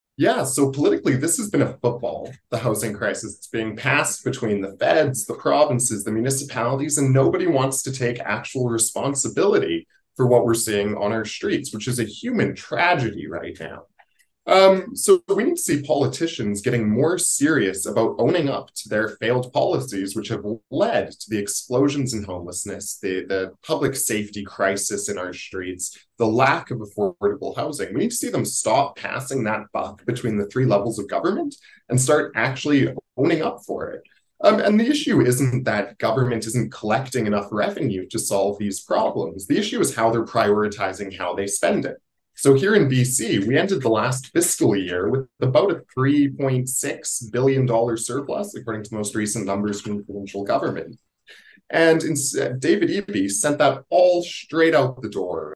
Carson Binda - Canadian Taxpayers Federation
While the City of Victoria wrestles with homelessness, drug addiction and mental illness on city streets the problem continues to simmer.  Carson Binda from the Canadian Taxpayers Federation says the weight of this problem shouldn’t be falling on civic shoulders and senior government must do it’s share.